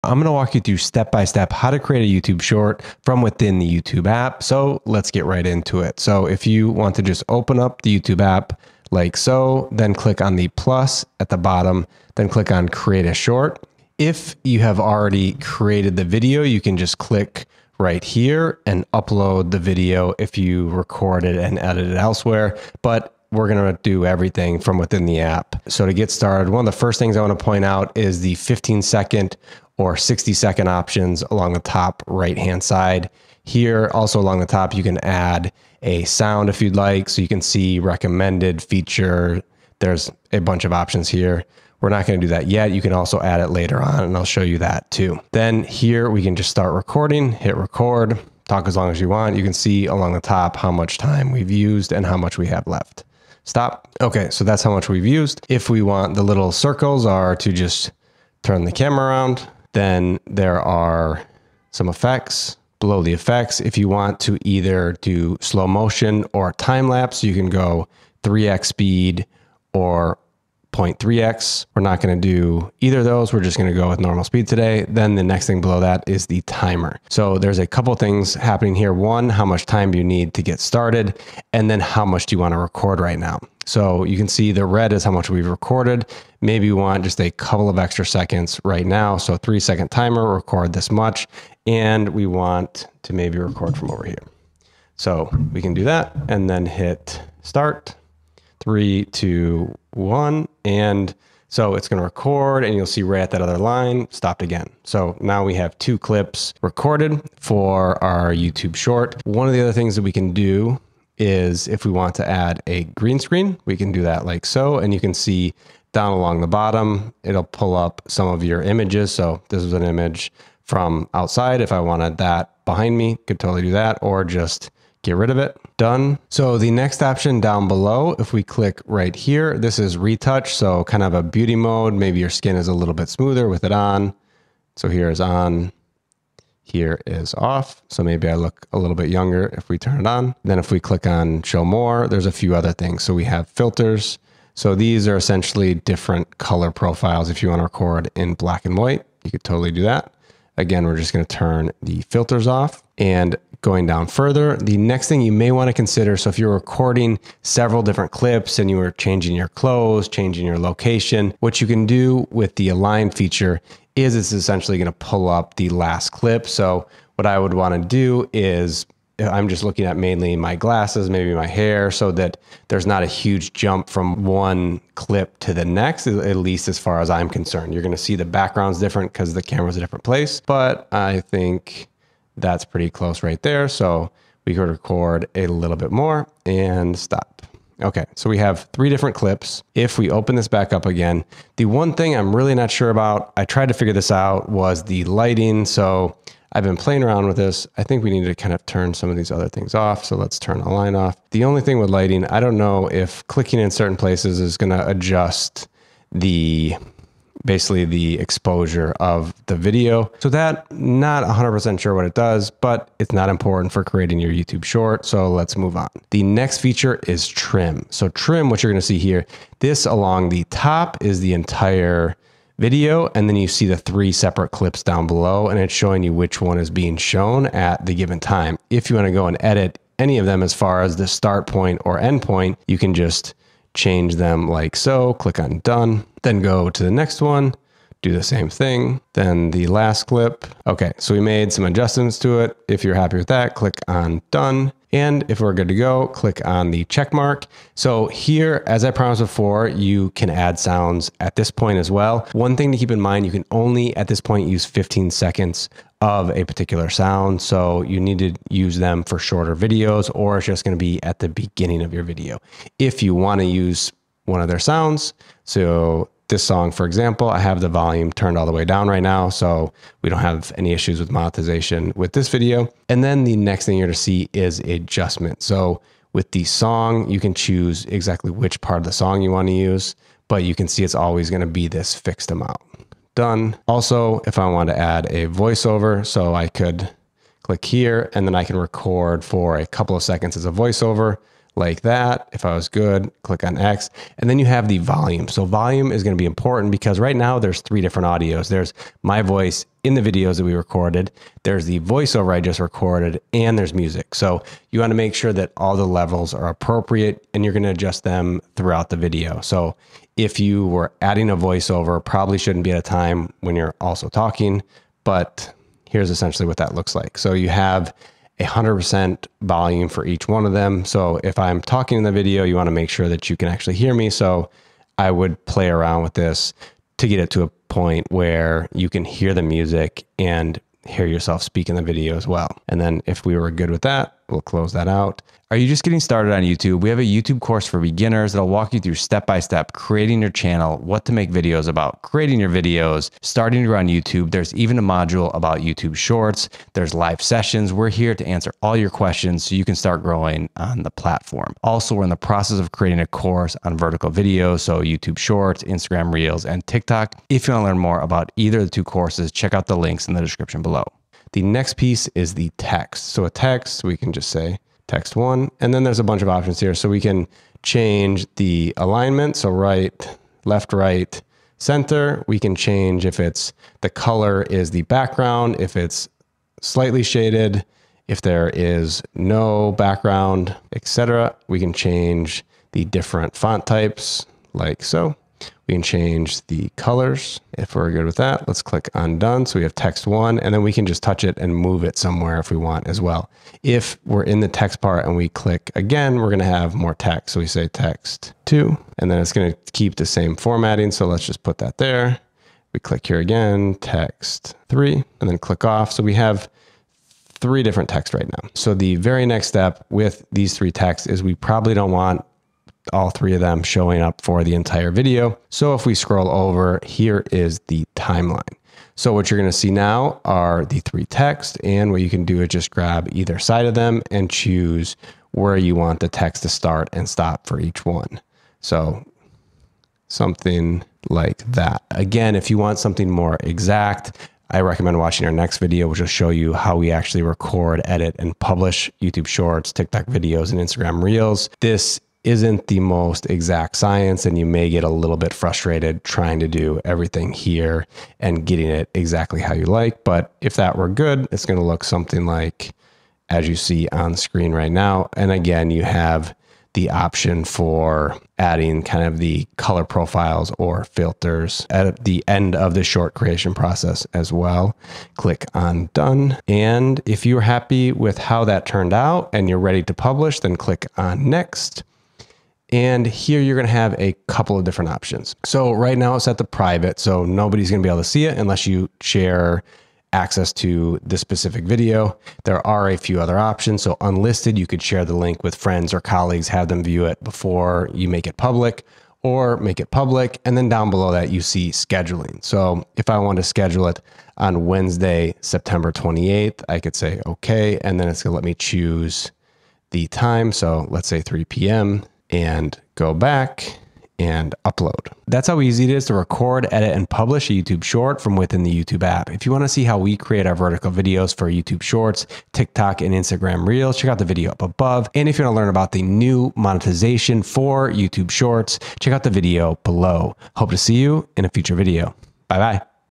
How to Make YouTube Shorts (Step by Step Tutorial for Beginners)
Here's a step by step guide on how to create a YouTube short from within the YouTube app. Follow this 2022 YouTube shorts tutorial to create your first short.